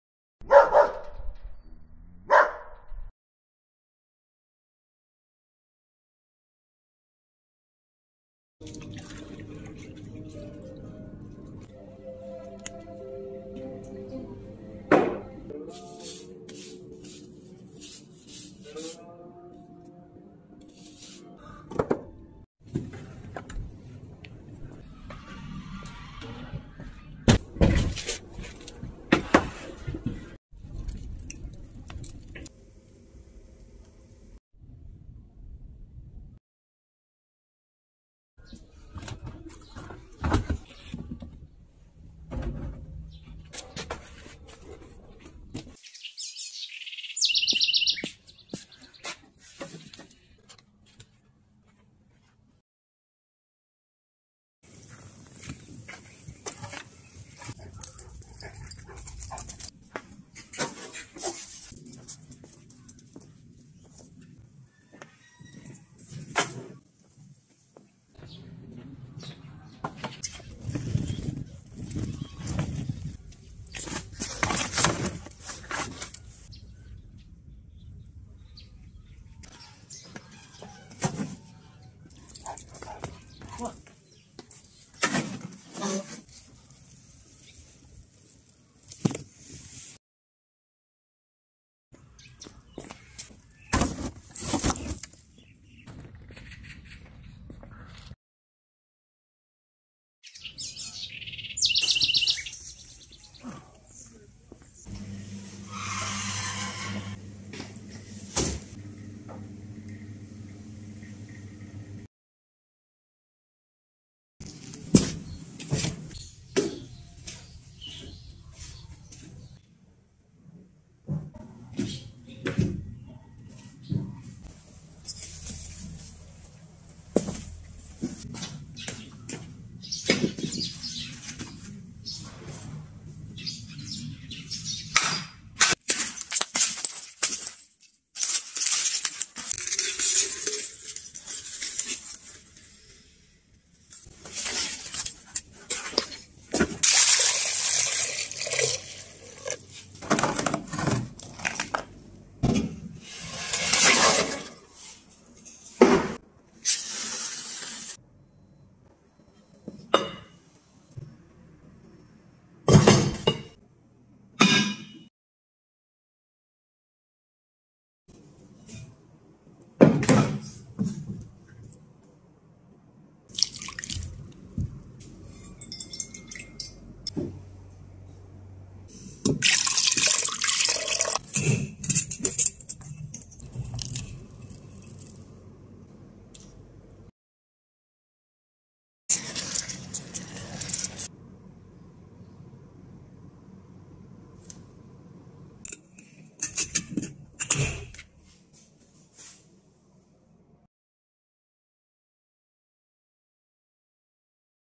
【九转膏（下）】古人美容减肥的方法，加入了九蒸九晒黄精，虽然制作过程复杂但吃了以后可以辟谷了！
泓雲子YouTube官方频道的一部分内容源于中国古籍记载，视频中的文字摘抄原文，不作为医学依据，复原古代饮膳服食，尊重自然，内心清静，无为的生活。